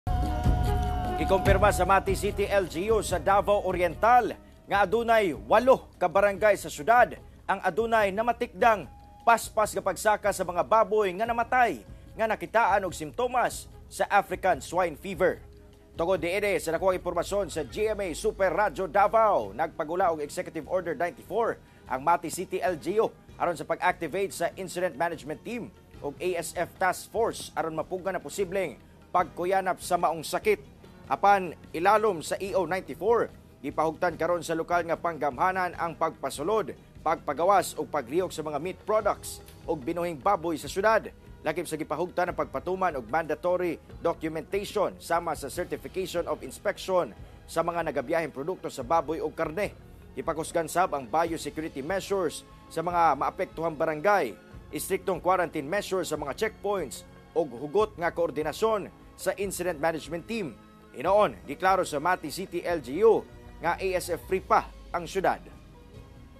One Mindanao: 8 ka barangay sa Mati City, gibantayan tungod sa pagkamatay sa mga baboy
8 ka barangay sa Mati City, Davao Oriental, gibantayan tungod sa pagkamatay sa mga baboy nga adunay simtomas sa ASF